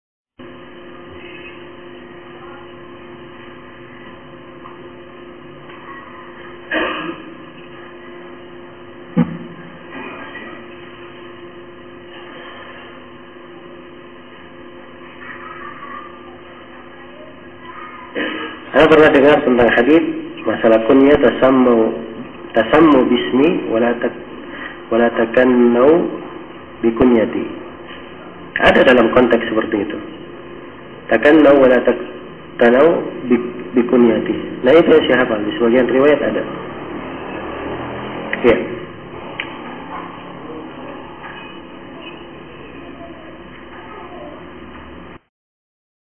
ilmoe.com D 2010 05 28 Btj02 Hadits Ttg Kunyah Abul Qasim
Judul : D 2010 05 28 Btj02 Hadits Ttg Kunyah Abul Qasim

Keterangan :
D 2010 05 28 Btj02 Hadits Ttg Kunyah Abul Qasim
Al Ustadz Dzulqarnain Dauroh Cepogo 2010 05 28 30